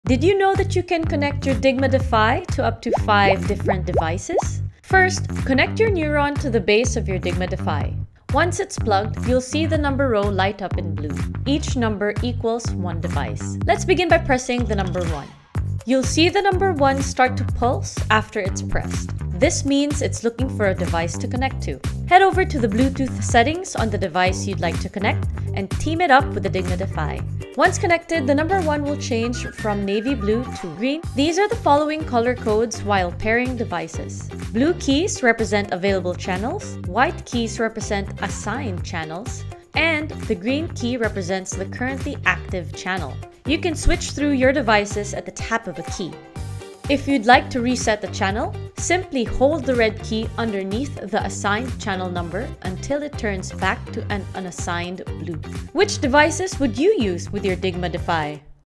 Seamlessly connect and switch between Bluetooth devices with your Dygma Defy! 📶
Use it as a media remote ✅ iPad keyboard ✅ or even a phone! ✅